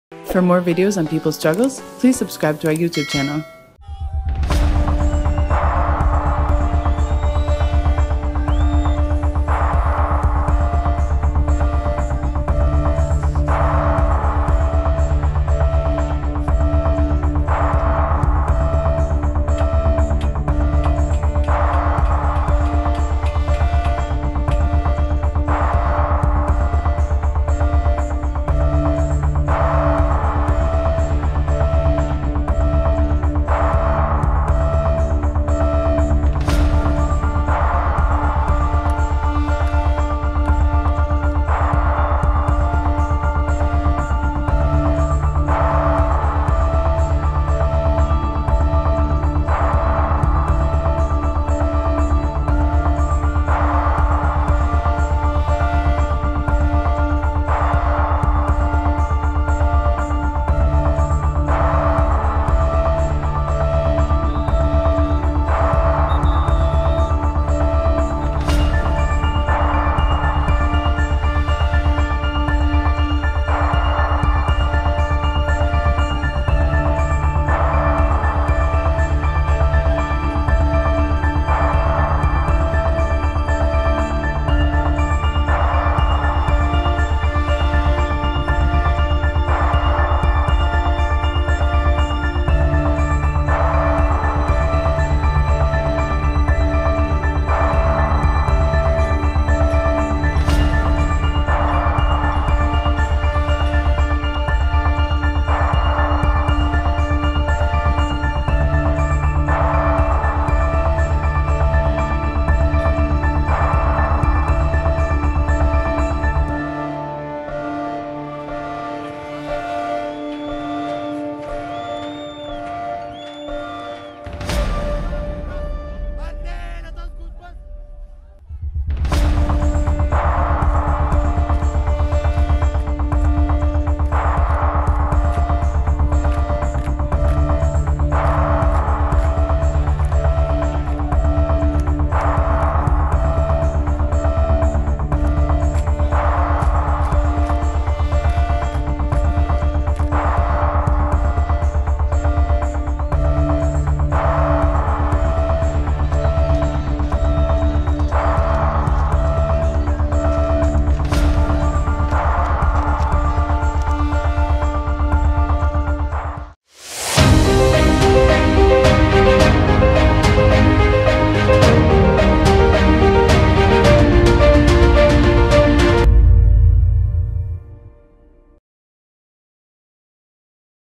Sudanese set to take to the streets again against military-Hamdok deal
Massive protests are set to take place in Sudan on Thursday against the deal struck by the military and deposed prime minister Abdalla Hamdok. Protesters have termed the deal – under which Hamdok will return at the head of a technocratic government – as a betrayal of the December Revolution which deposed former dictator Omar al-Bashir.